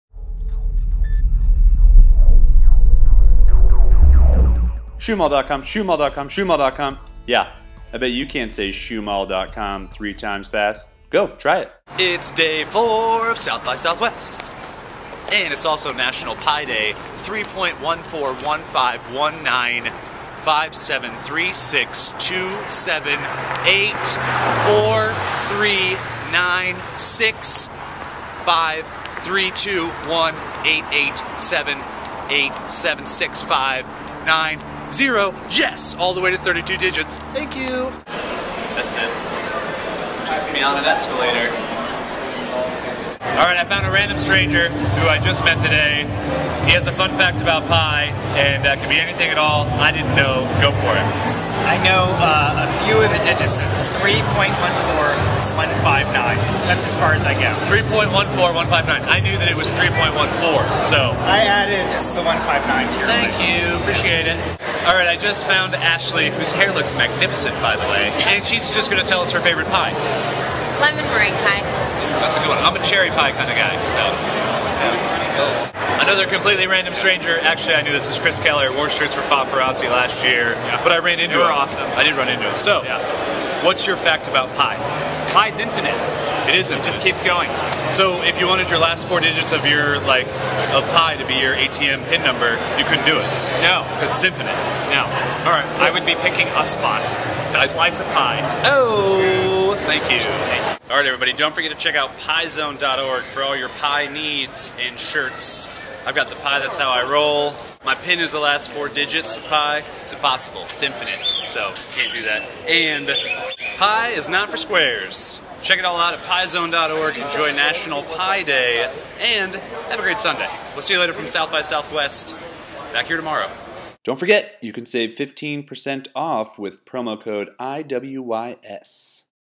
Day 4 of SXSW and it's Pi Day
Do you love Pi? Do you know much about the number Pi? The number 3.14 is crazy important and shows up everywhere in the world, especially at SXSW.